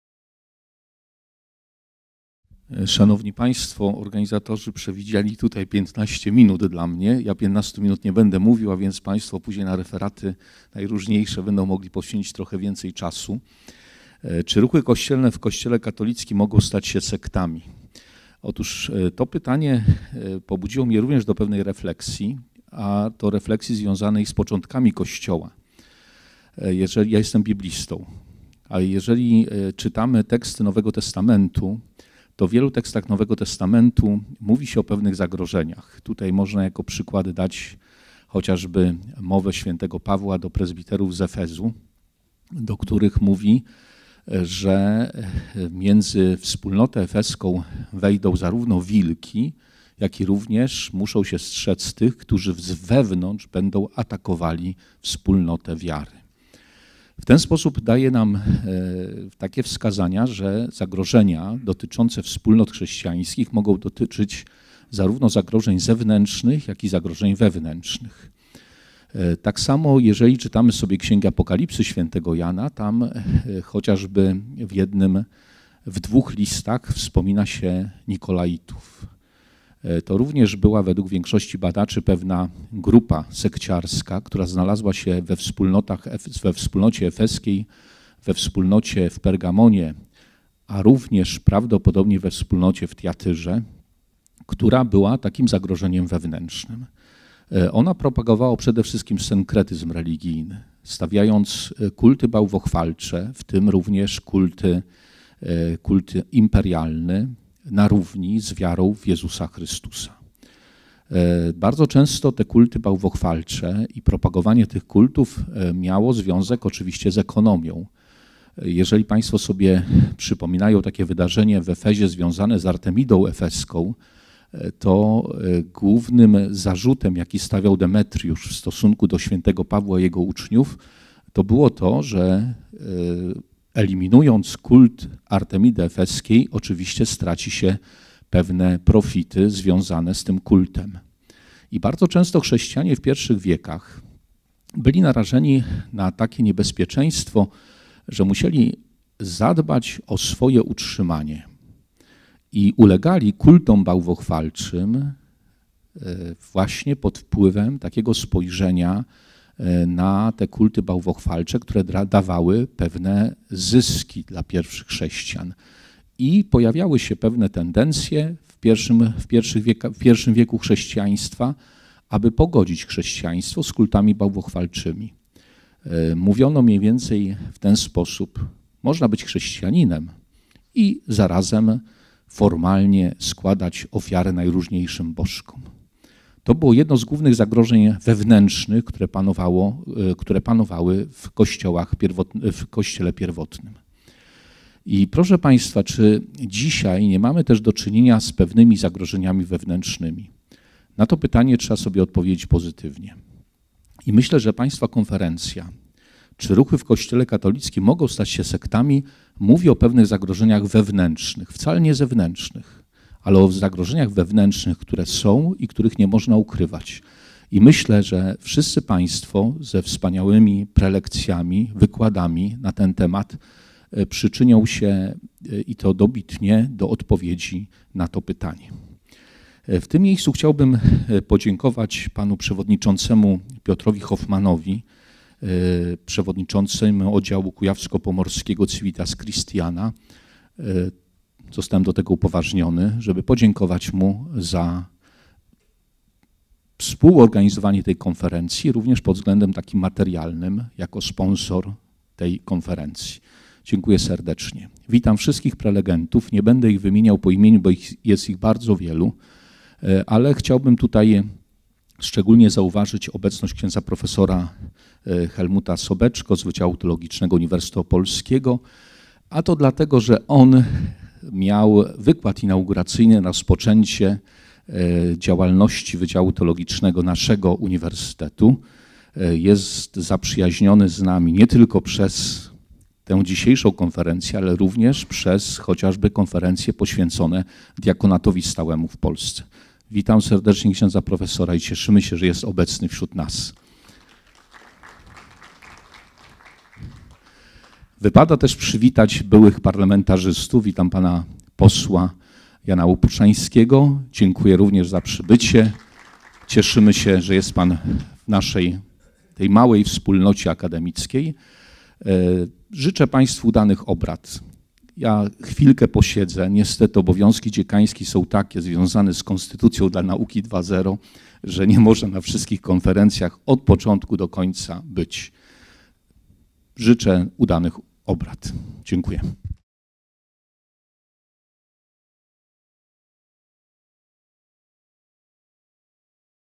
Otwarcie X Toruńskie Sympozjum poświęcone Religiom i Alternatywnym Ruchom Religijnym. Ks. D. Kotecki
CZY RUCHY W KOŚCIELE KATOLICKIM MOGĄ STAĆ SIĘ SEKTAMI?
X Toruńskie Sympozjum poświęcone Religiom i Alternatywnym Ruchom Religijnym
Toruń, 15 listopada 2018
Otwarcie konferencji
Ks. prof. dr hab. Dariusz Kotecki
– Dziekan Wydziału Teologicznego UMK